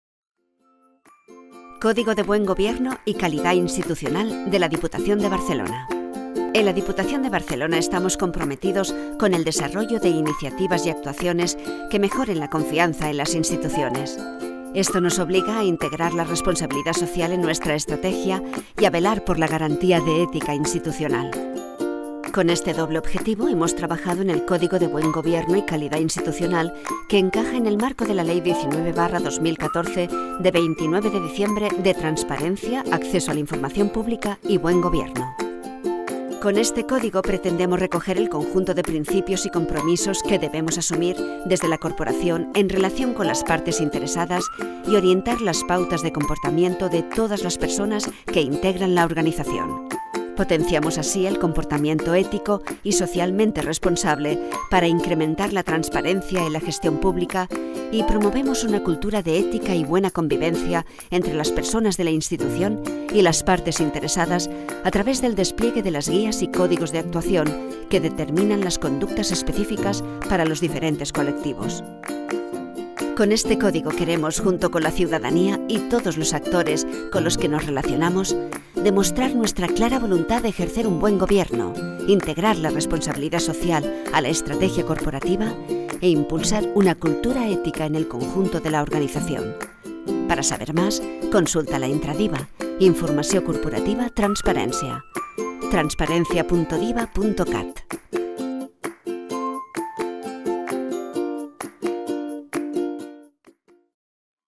Código de buen gobierno y calidad institucional
Código de buen gobierno y calidad institucional es una declaración firme de compromiso de la Diputación como institución con ética pública y la transparencia que, entre todos, debemos trasladar a la práctica del trabajo diario.